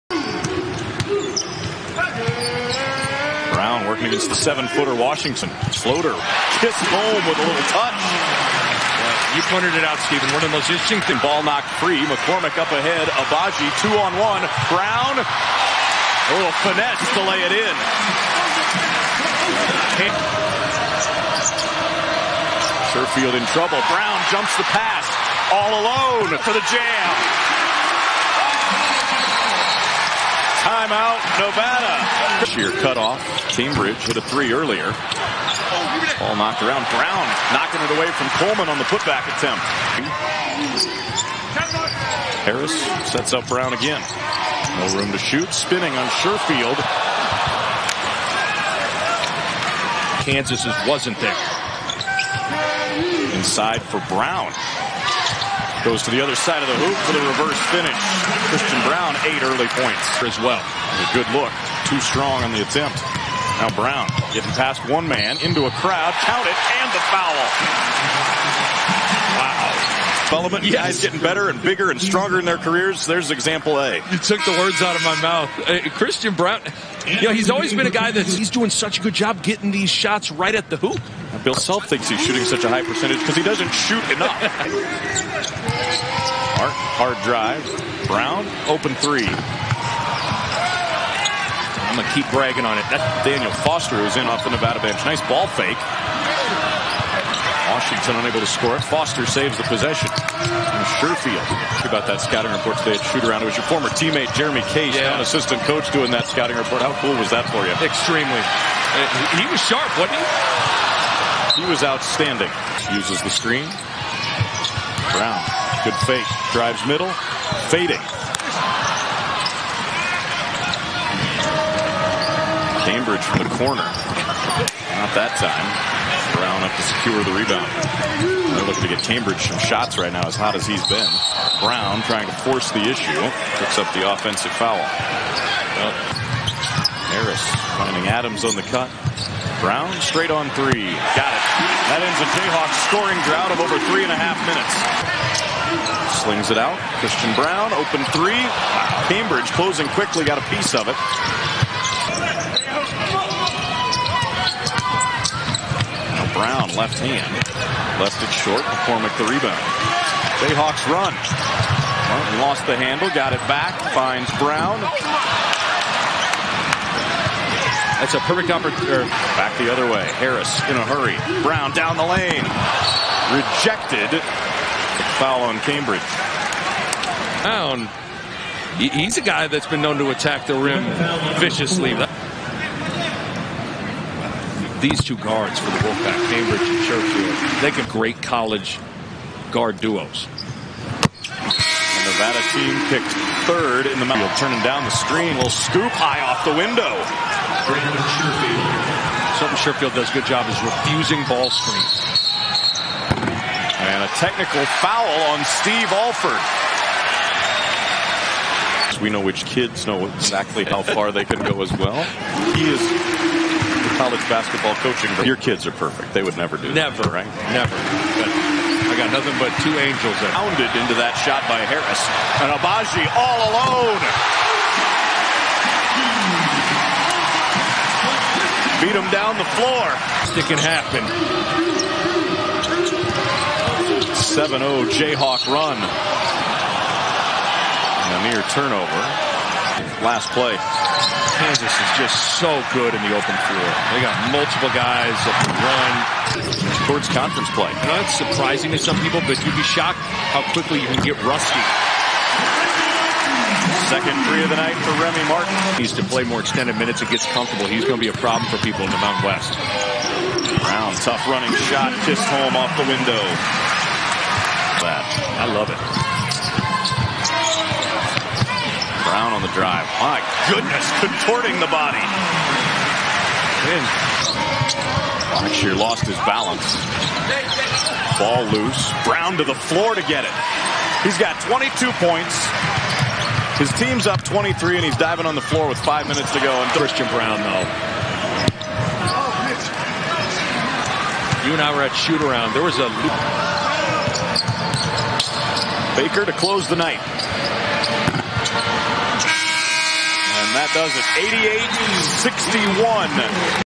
Christian Braun vs Nevada 12.29.21 | 22 pts (8-14 FG, 1-3 3pt, 5-5 FT), 4 Reb, 2Ast, 2Stl, 2Blk, 1TO
Every shot, assist, rebound, steal, block, & turnover from 6'7" 218 lbs Kansas guard Christian Braun's 12-29-21 game against Nevada. Get the FULL picture, not just the highlights.